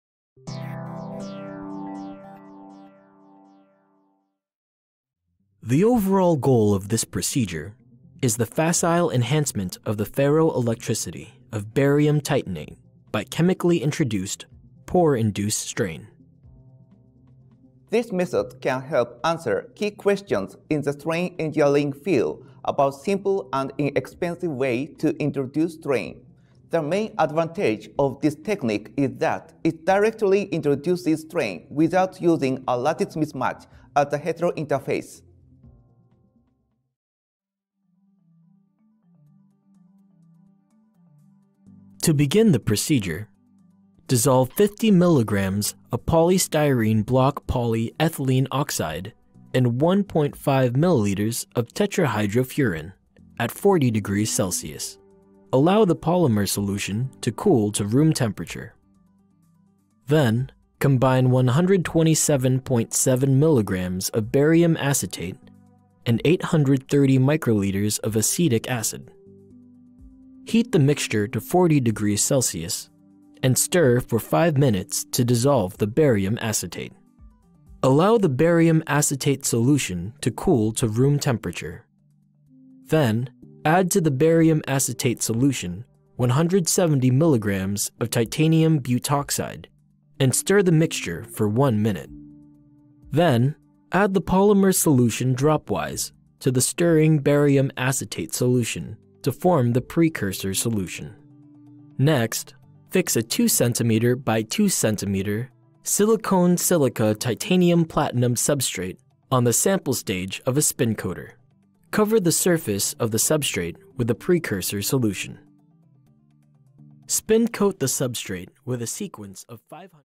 Porous Barium Titanate Thin Film & Thermal Stabilization-Ferroelectric Phase l Protocol Preview
Chemical Synthesis of Porous Barium Titanate Thin Film and Thermal Stabilization of Ferroelectric Phase by Porosity-Induced Strain -  a 2 minute Preview of the Experimental Protocol

Norihiro Suzuki, Minoru Osada, Motasim Billah, Yoshio Bando, Yusuke Yamauchi, Shahriar A. Hossain
Tokyo University of Science (TUS), Research Institute for Science and Technology (RIST); National Institute for Materials Science (NIMS), International Center for Young Scientists (ICYS); National Institute for Materials Science (NIMS), International Center for Materials Nanoarchitectonics (MANA); University of Wollongong, Australian Institute for Innovative Materials (AIIM); University of Queensland, School of Chemical Engineering, Australian Institute for Bioengineering and Nanotechnology (AIBN); Kyung Hee University, Department of Plant & Environmental New Resources;

Here, we present a protocol for the synthesis of porous barium titanate (BaTiO3) thin film by a surfactant-assisted sol-gel method, in which self-assembled amphipathic surfactant micelles are used as an organic template.